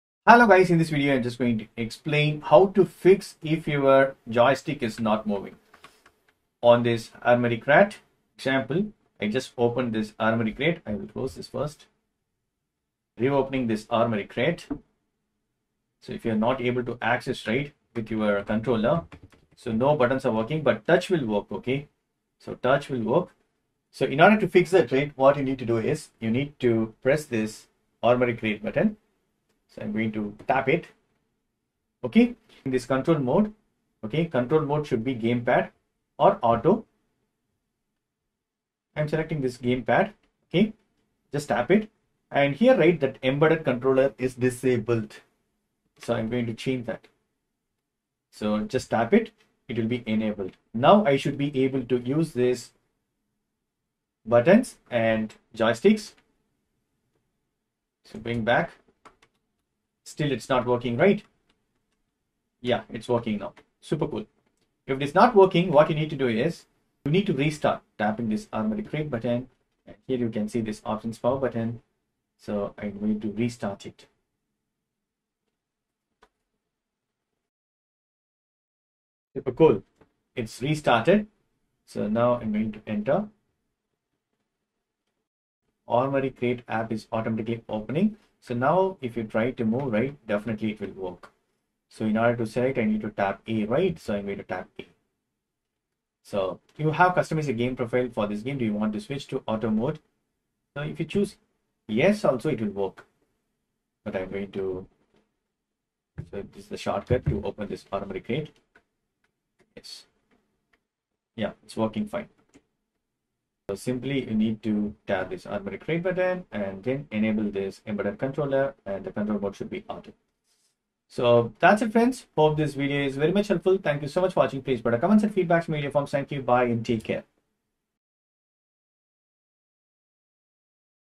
{Fixed} Asus ROG Ally Joystick Not Working in Armoury Crate Game Library
Hello guys, in this video, i am explaining how to fix asus rog ally joystick not working in Armoury Crate Game Library.